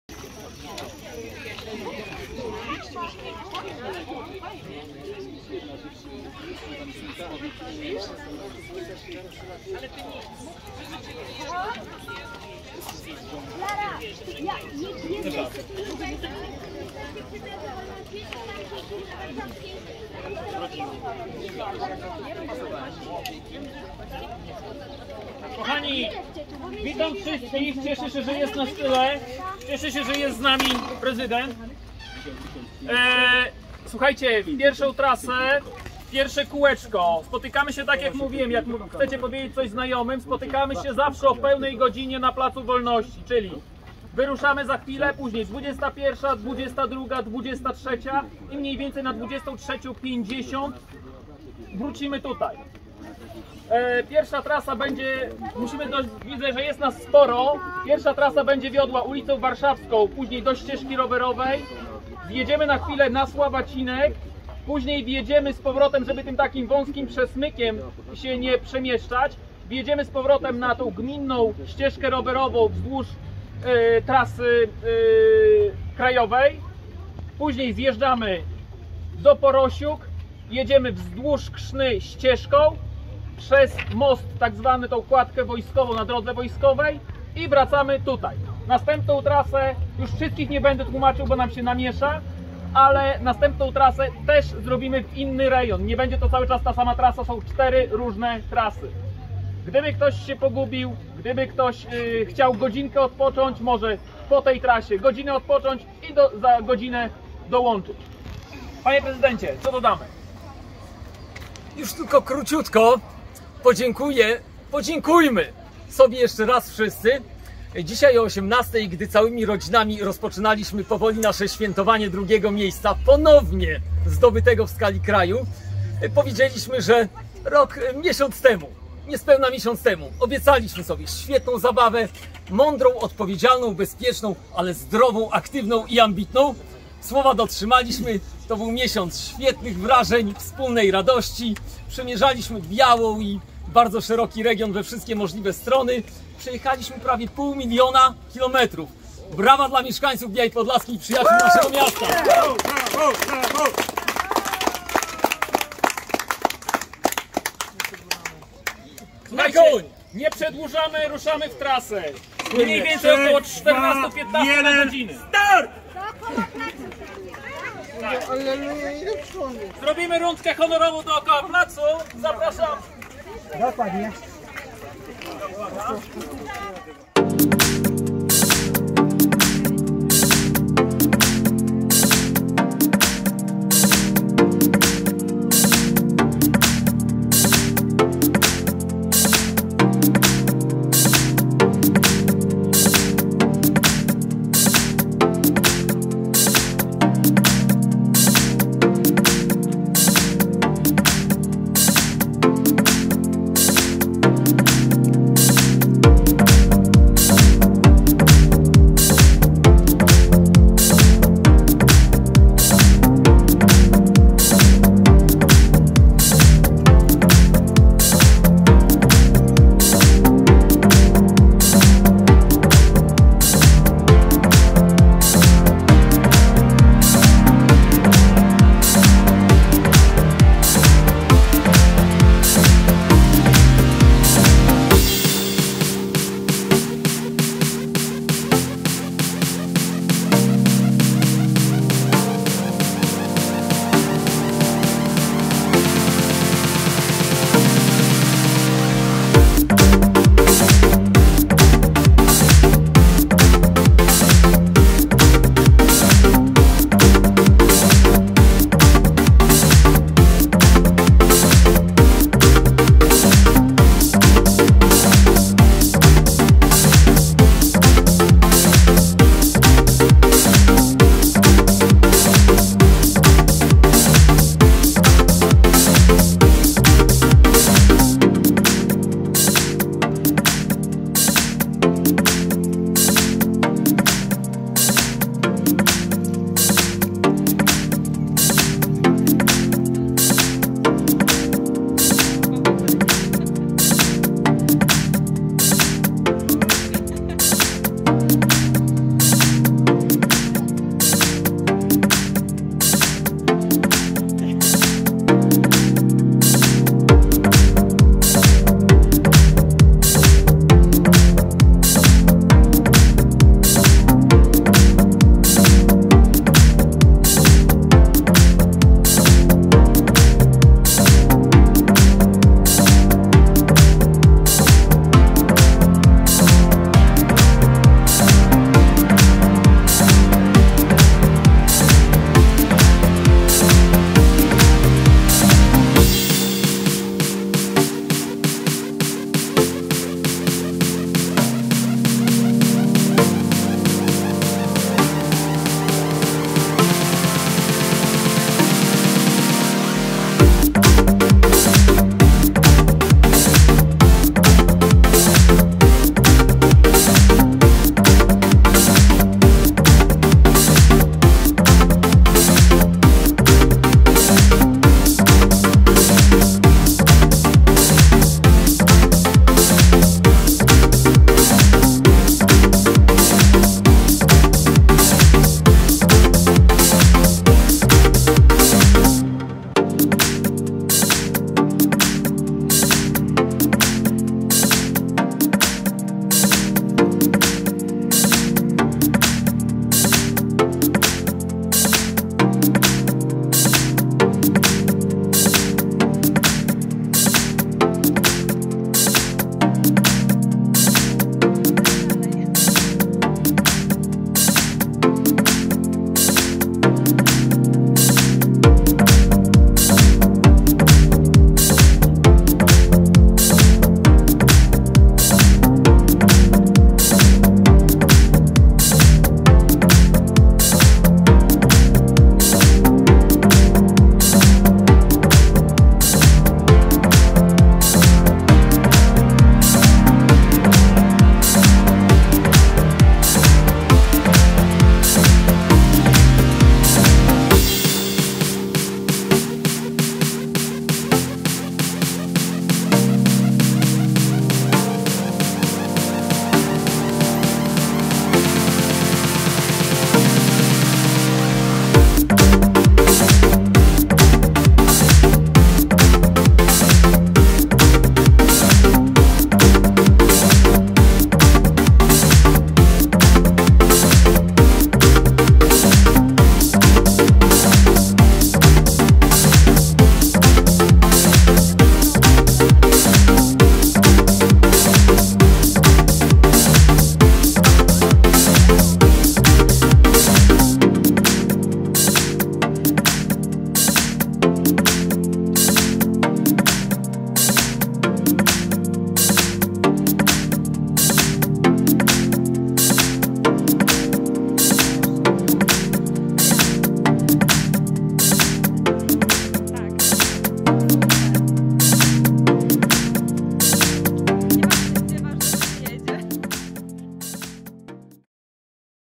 Biała Podlaska walczy do samego końca - ostatnie 4 godz. rywalizacji o Rowerową Stolicę Polski!
Od godziny 20:00 z placu co godzinę wyrusza rajd, planowane są 4 przejazdy ok. 14 km na 4 różnych trasach, po różnych częściach miasta.  Chętnych zapraszamy o 21:00, 22:00 i 23:00. Ok. 23: 50 na placu planowane jest spotkanie podsumowujące tegoroczny puchar o Rowerową Stolicę Polski. Wszystko wskazuje na to, że obronimy drugie miejsce :)